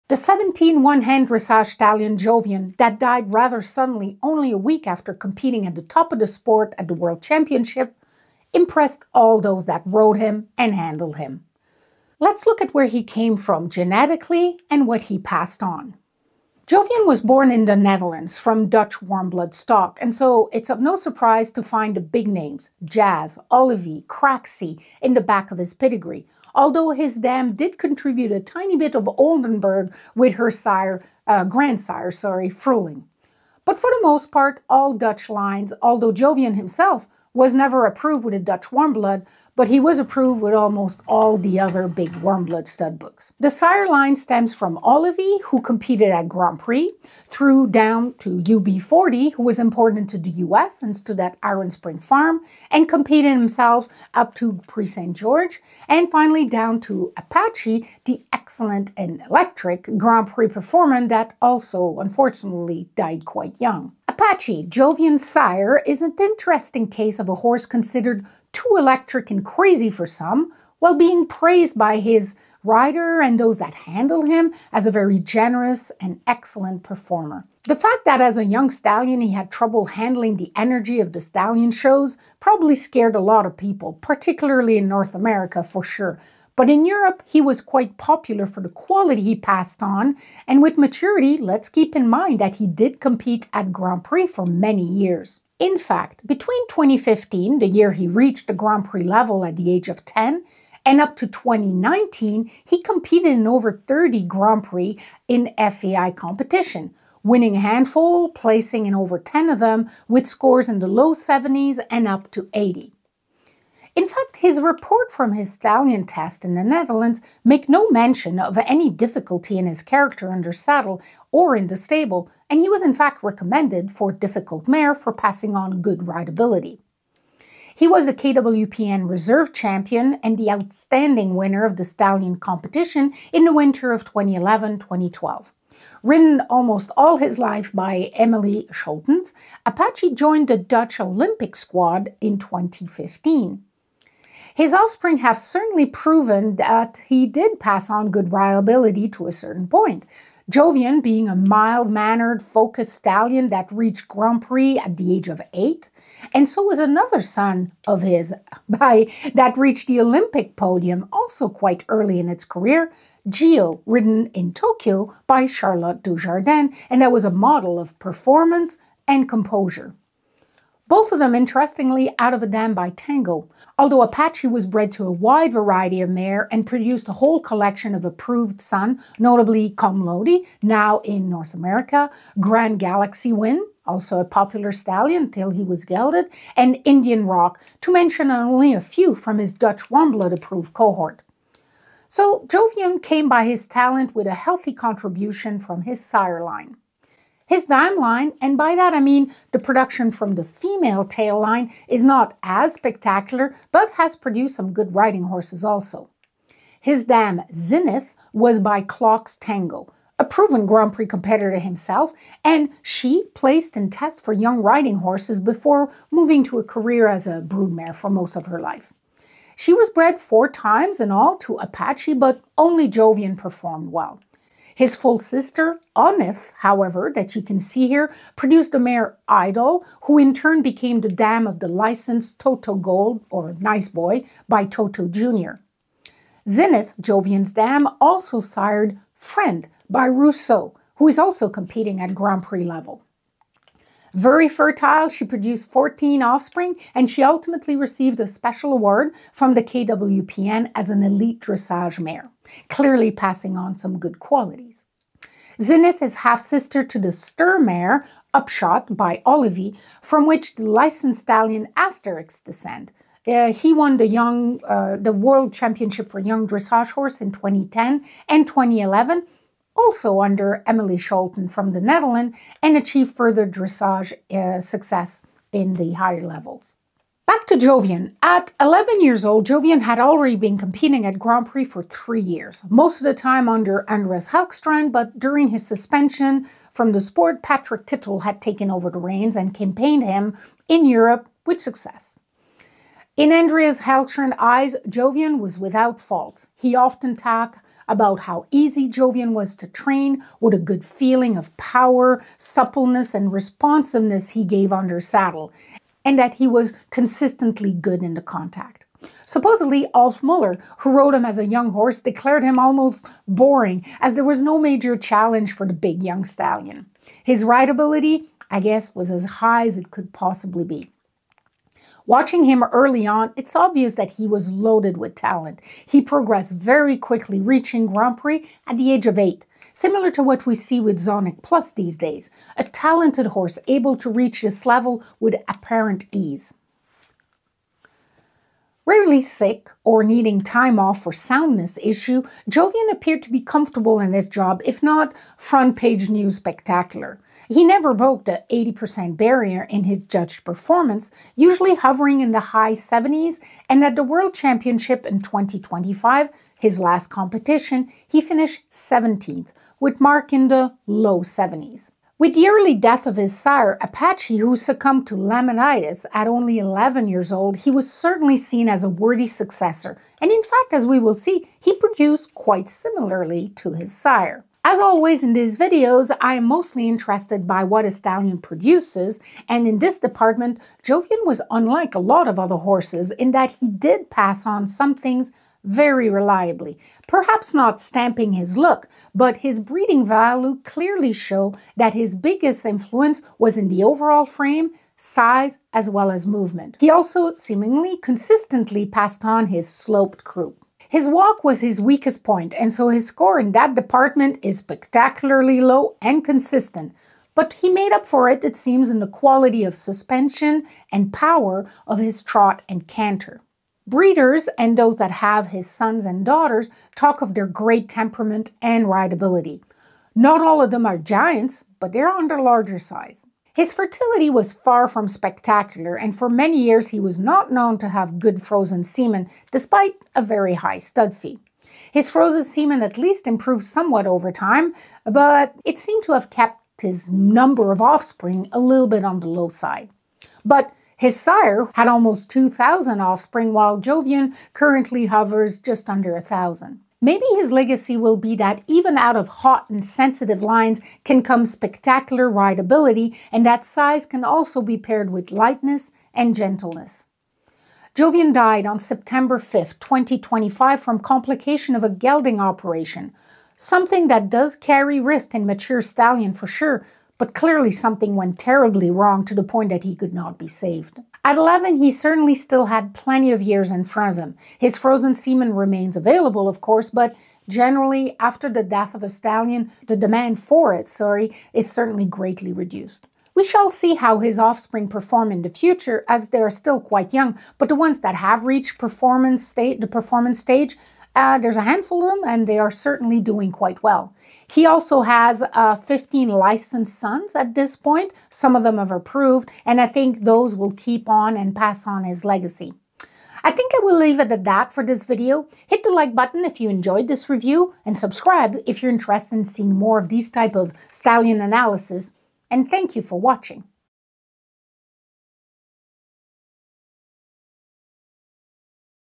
Jovian: His heritage and legacy
Gone too soon this video is about the pedigree behind Jovian and what he passed on to the next generation.

⏰ Timecodes ⏰
00:00 - Title of Chapter